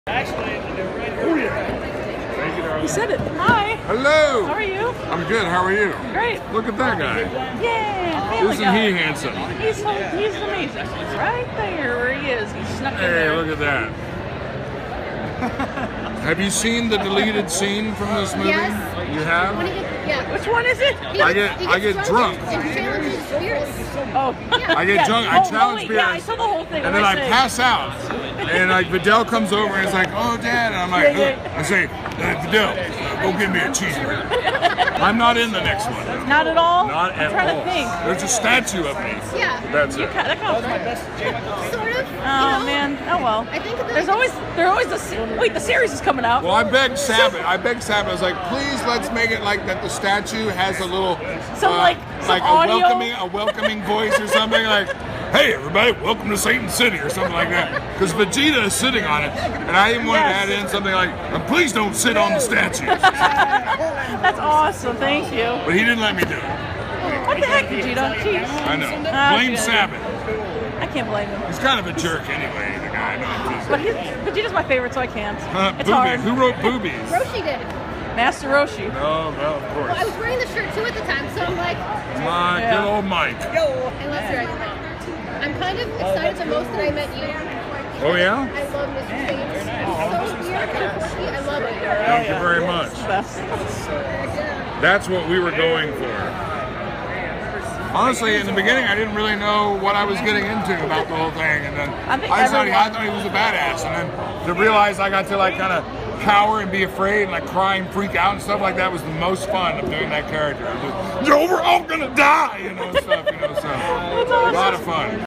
Meeting Chris Rager at Anime Midwest 2015
Meeting finally THE CHAMP HIMSELF Chris Rager! Voice of Mr. Satan (Hercule) in Dragon Ball Z at Anime Midwest 2015!  Listen to his awesome story that I personally think should of happened in Resurrection F! My friend Brittany was also with me meeting him.